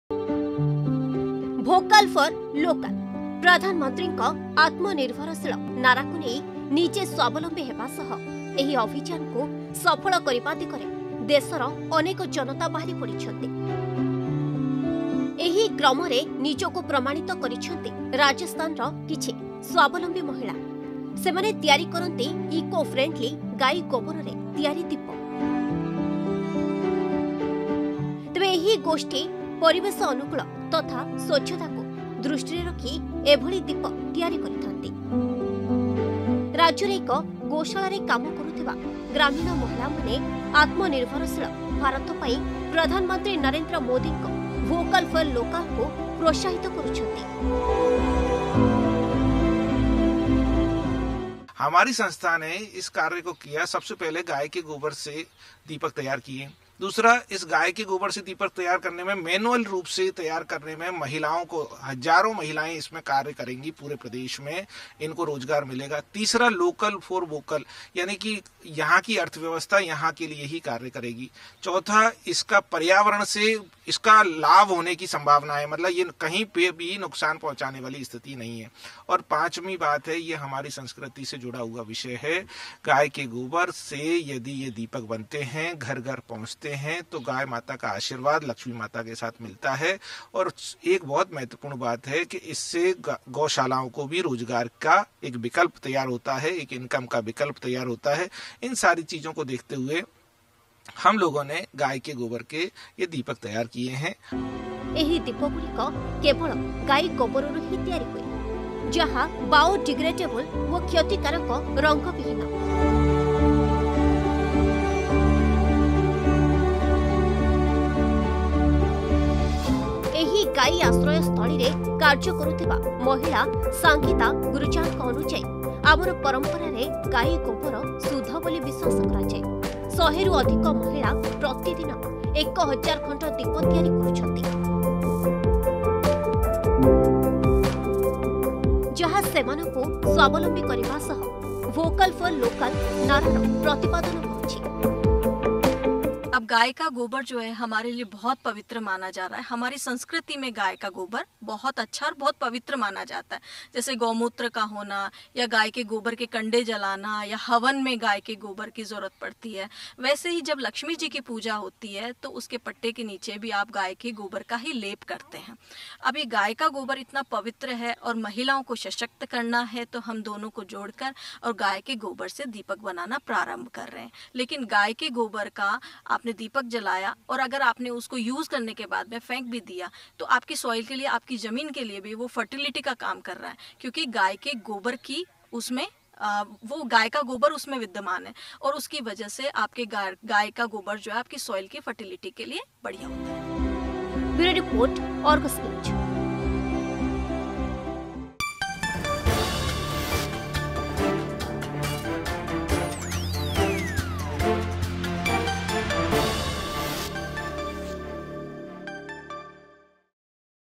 ଗାଈ ଗୋବରୁ ତିଆରି ହେଉଛି ଦୀପ
ସ୍ୱାବଲମ୍ବୀ ମହିଳାଙ୍କ ଭୋକାଲ ଫର ଲୋକଲ କାମ । ନିଜକୁ ପ୍ରମାଣିତ କରିଛନ୍ତି ରାଜସ୍ଥାନର କିଛି ସ୍ୱାବଲମ୍ବୀ ମହିଳା । ଗାଈ ଗୋବରରୁ ପ୍ରସ୍ତୁତ କରୁଛନ୍ତି ସୁନ୍ଦର ଦୀପ...